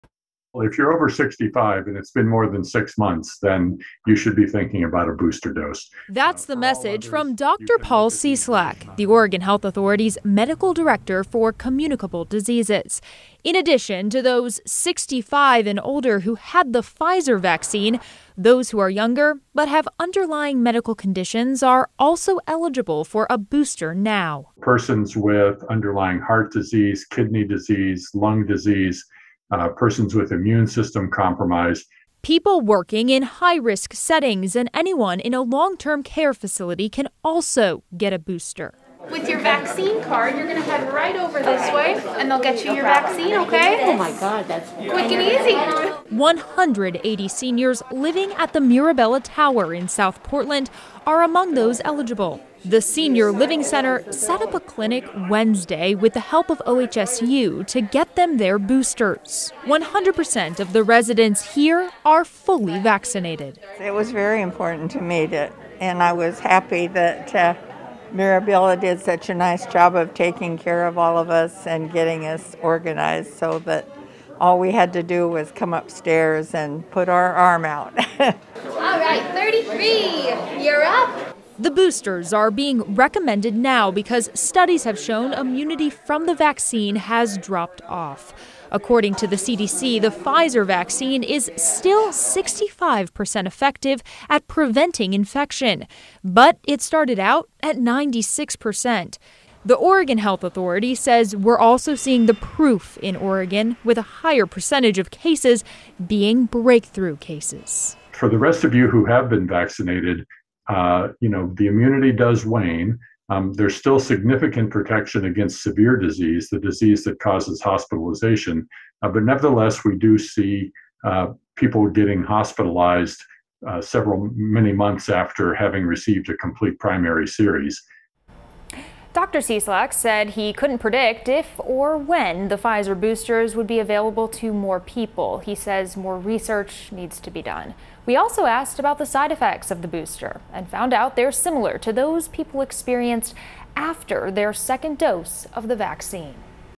Here's who should get a COVID-19 vaccine booster shot
With vaccine immunity waning, doctors say people 65 and older or those with underlying health conditions should get a booster. KGW's Alma McCarty reports.